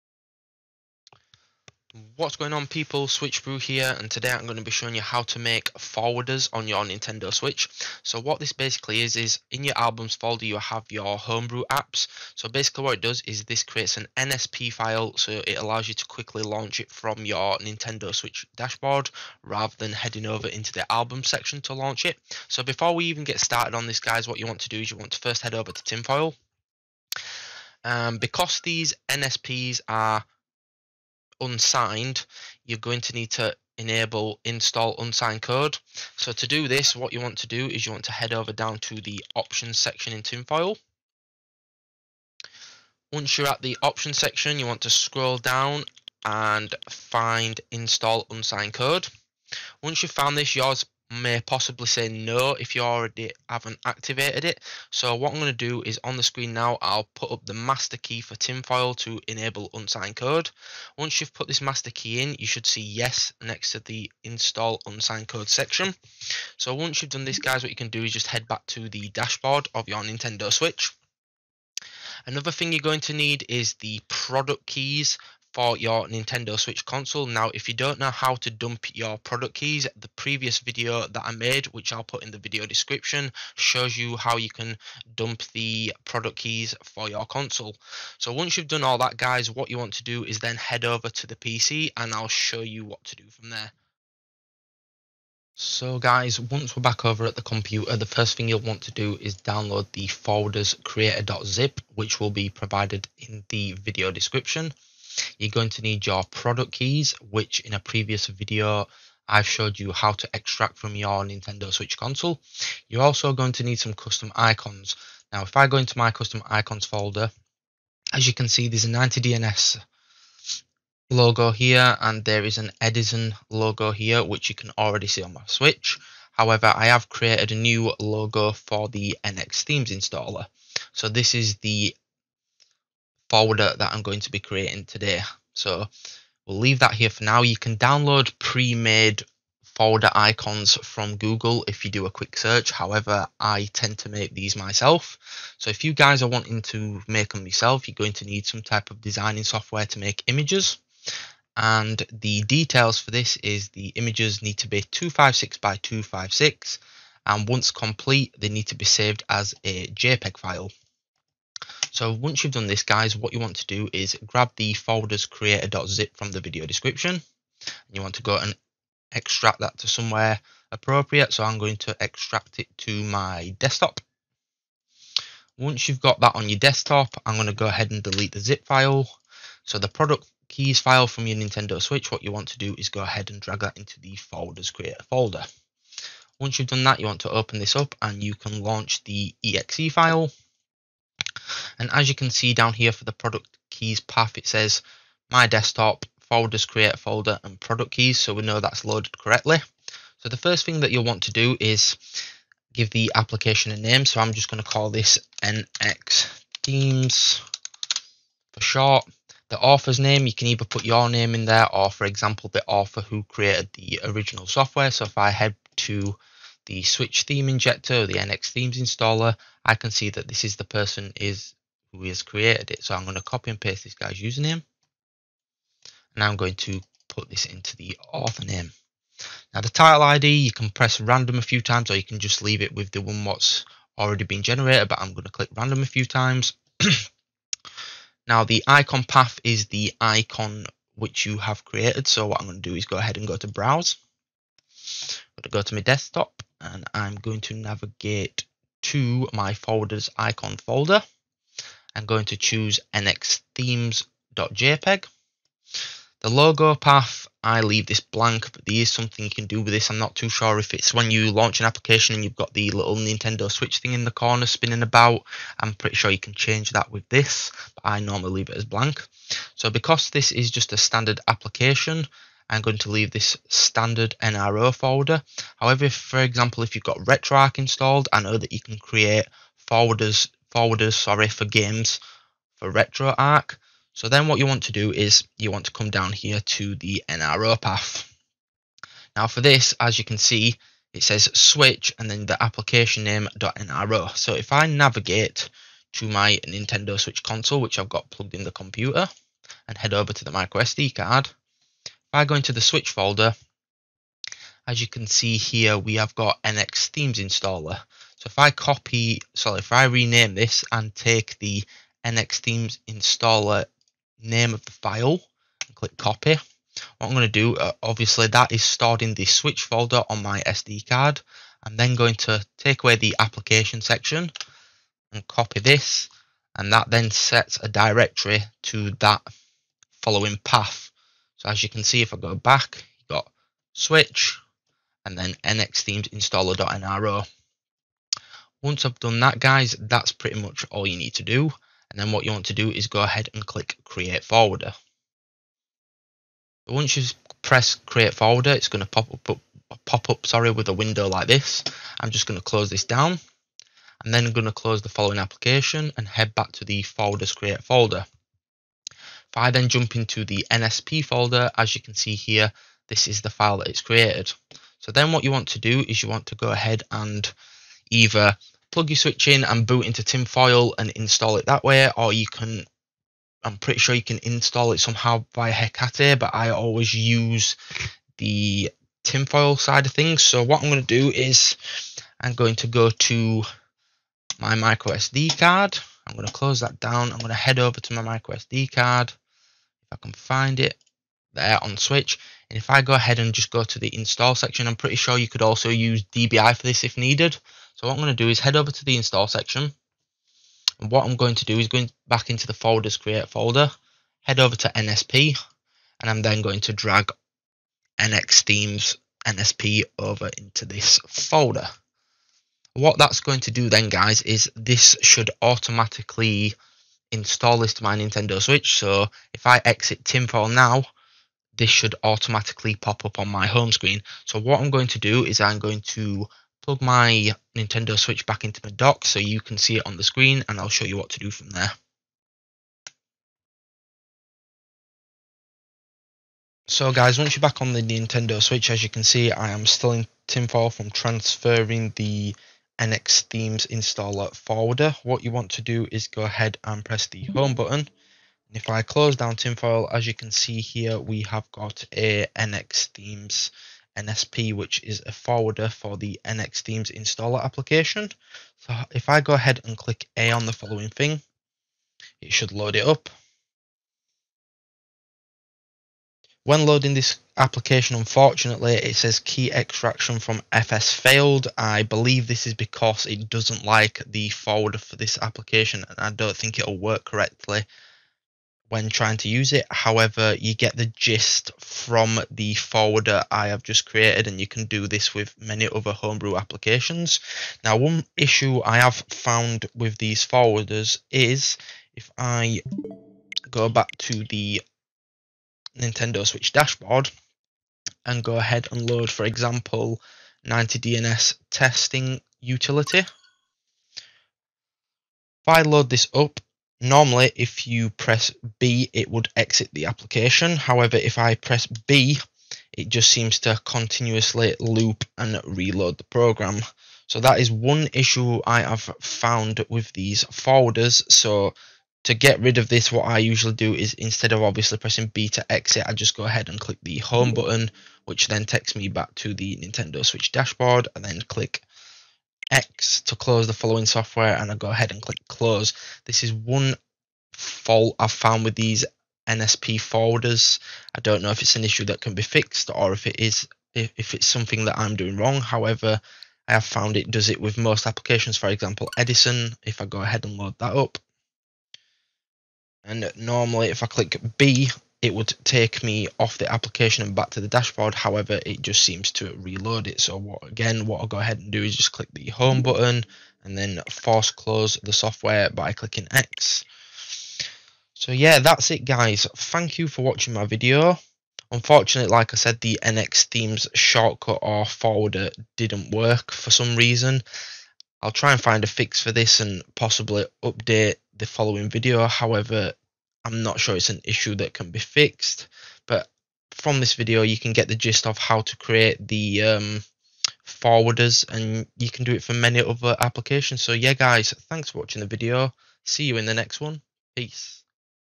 How To Create Hombrew Forwarder's Nintendo Switch 2023!
Todays Video I Will Be Showing You How To Create Homebrew Forwarder's For Your Nintendo Switch Console.

⬇️Add Me On Discord For 1 To 1 DM's And Help⬇️
🌏switchbrew

☲☲ Links As Described In The Following Video ☲☲